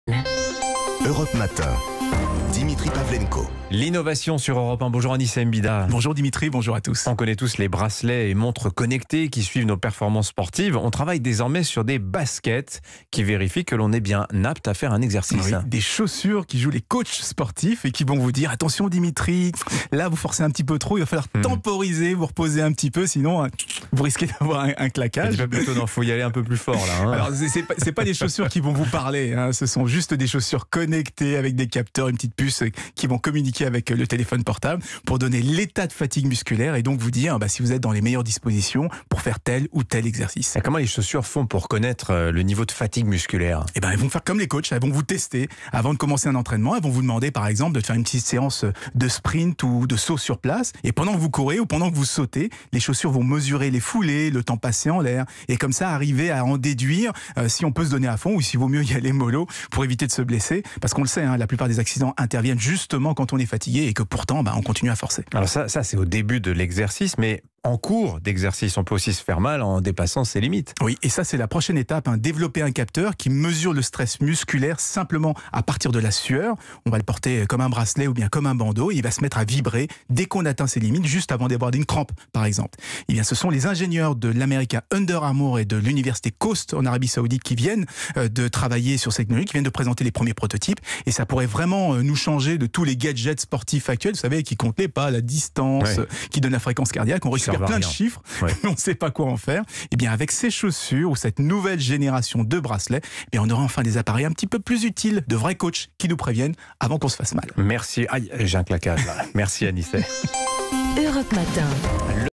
Sport : des baskets qui vérifient que l’on est bien apte à faire un exercice
Chaque jour, Anicet Mbida nous fait découvrir une innovation qui pourrait bien changer notre façon de consommer. Ce lundi, il s'intéresse à une nouveauté dans le domaine du sport, des baskets qui vérifient que l’on est bien apte à faire un exercice.